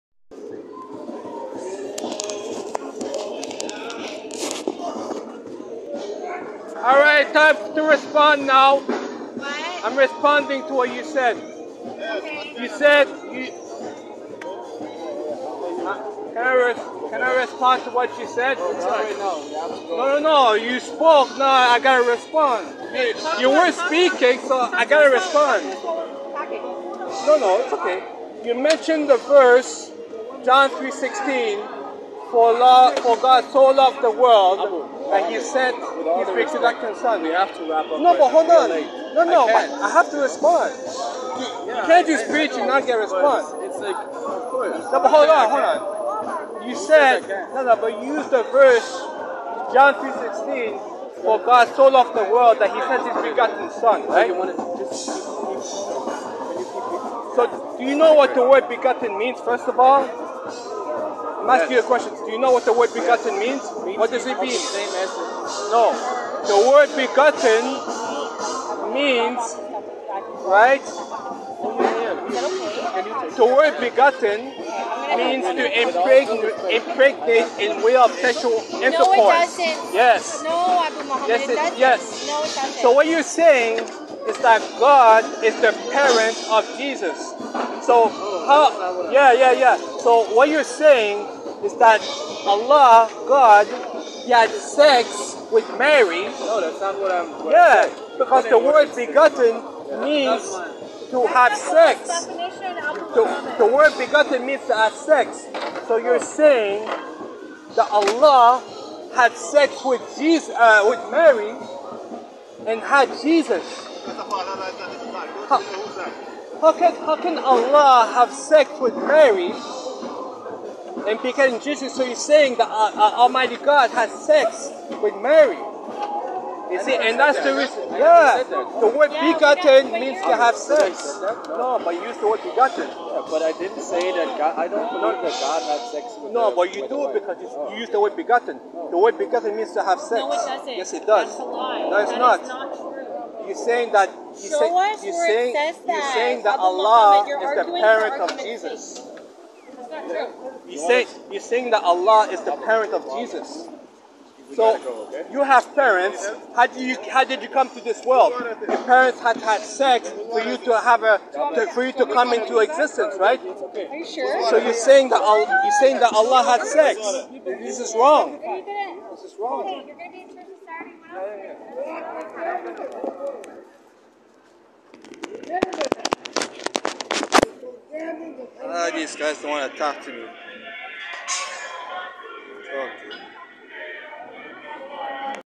Muslim response to a German Christian's stating John 3:16 "For God so loved the World..."
Abu-Muhammad Kamena(Former Christian)was in downtown Toronto when he heard a Christian Evangelist preaching that "Jesus is the son of God" by mentioning the famous John 3:16 verse.He tried to explain to the Christian preacher that the word " begotten " means to impregnate in way of sexual intercourse"and the lady at the back tried to deny the definition.